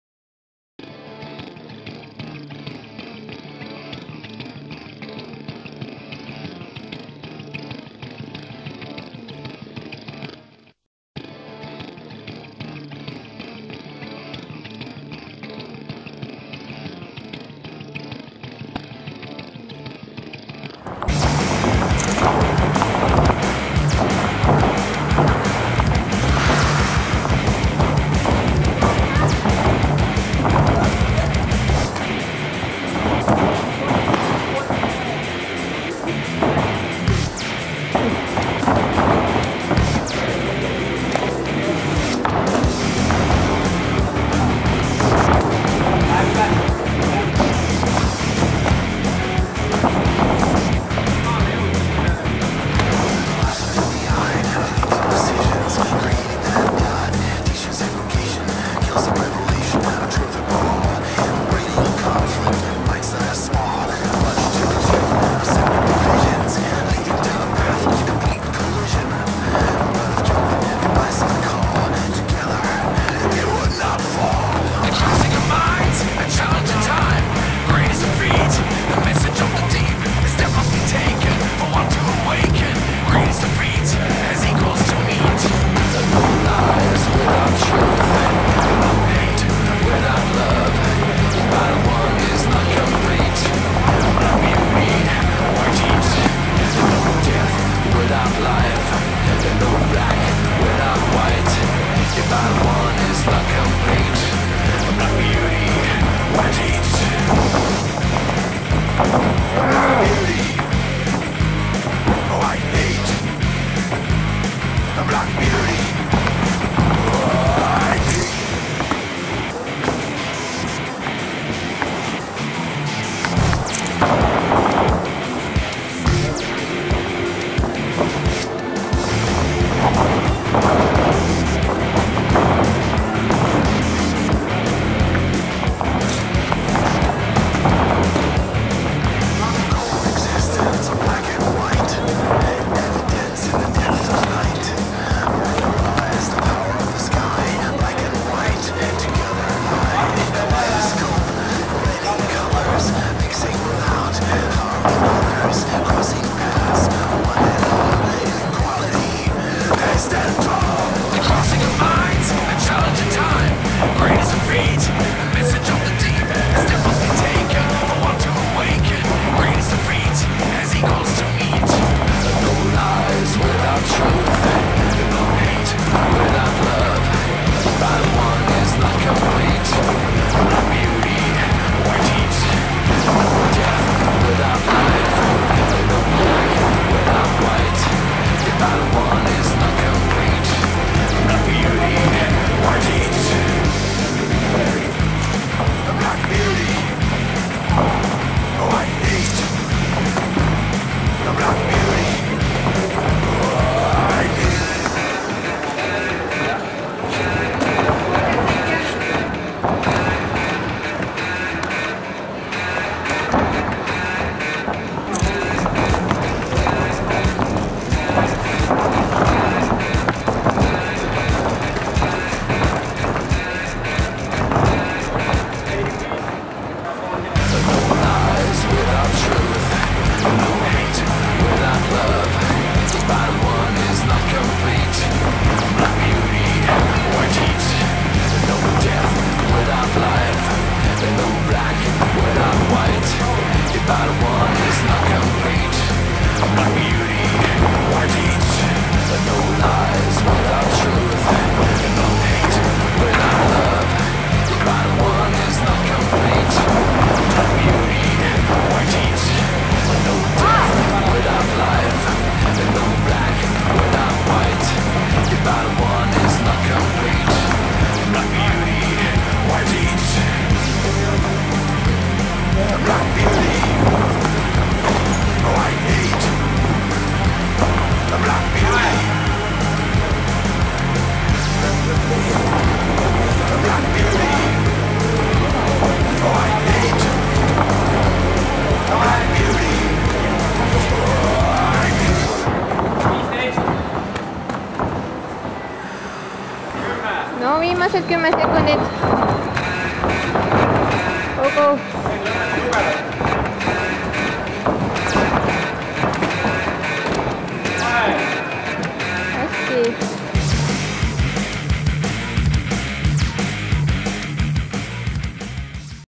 RAJU 14 demopäeva puurisparr
Spordiklubi Kombat peatreener Rain "Sifu" Ruuder käis 4. oktoobril 2014 RAJU 14 finaalsündmuse demopäevadel Tartu Lõunakeskuses Raju-võitlejatega tibake "müramas" ja jõudu katsumas!